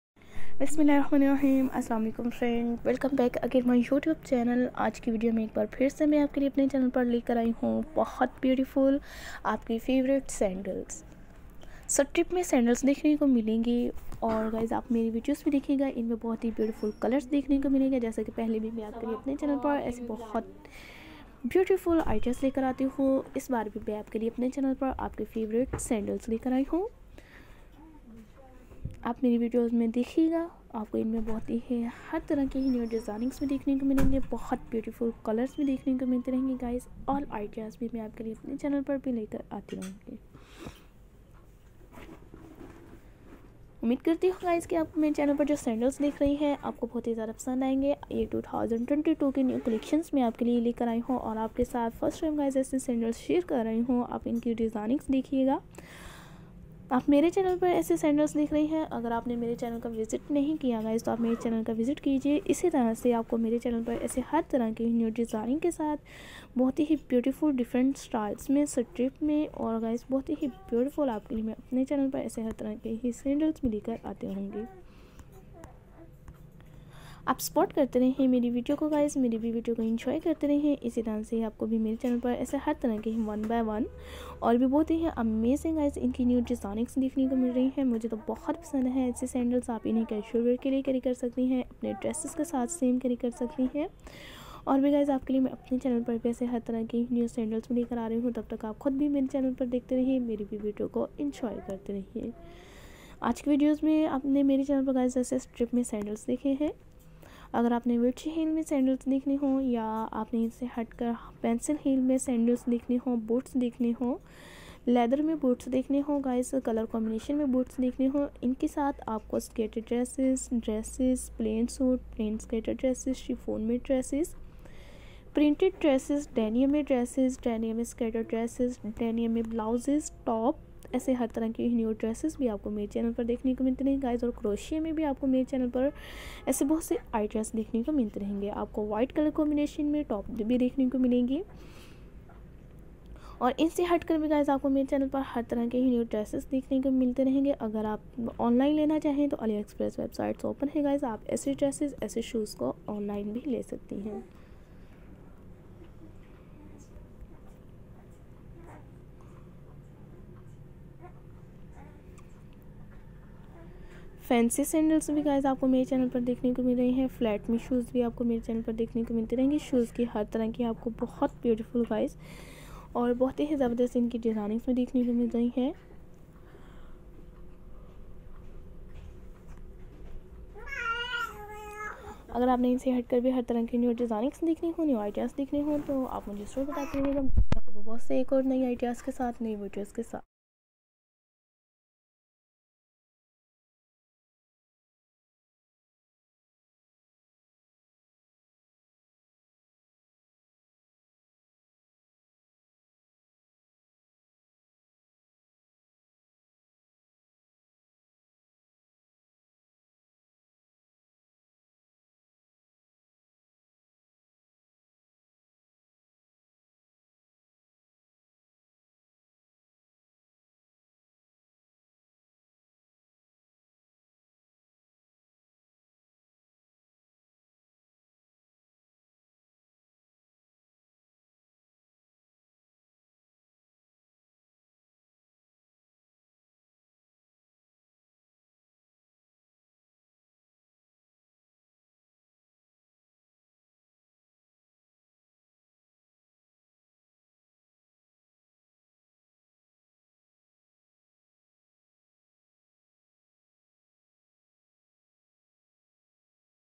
Top65 pure leather open toe sling back side buckle mid heal comfortable women casual wear sandals
Top 65running & stylish pure leather open toe block mid heels casual wear striped style sandals